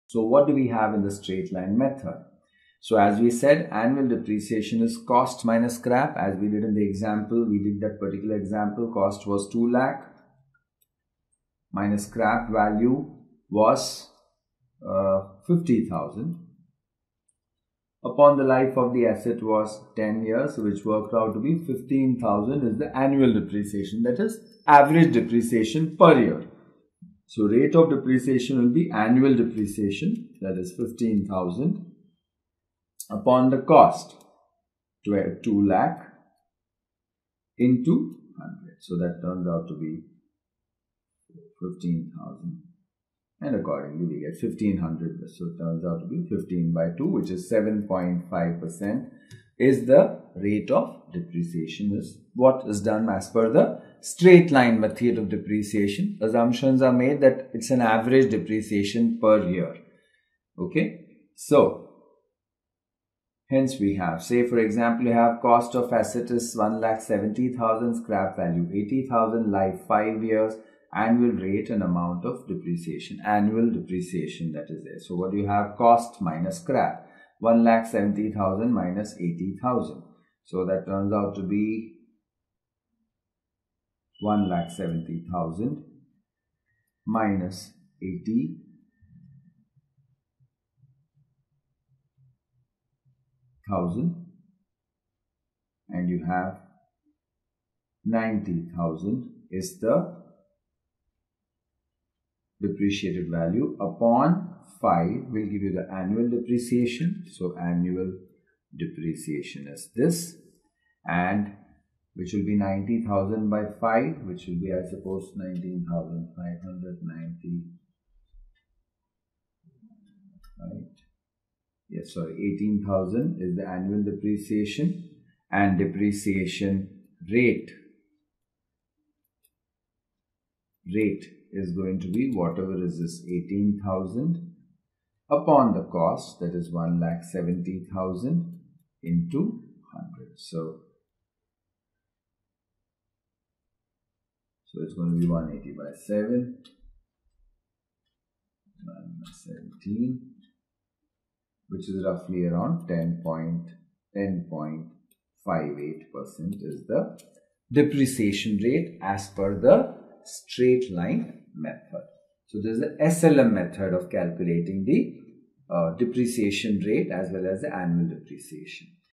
Straight Line Method | Interest | Tutorials | CA CPT | CS & CMA Foundation | Class 11 | Class 12
Straight Line Method, Learn Basics of Interest, Simple Interest, Compound Interest, Nominal vs. ERR, What is Annuity? What is Perpetuity? Problems on Interest.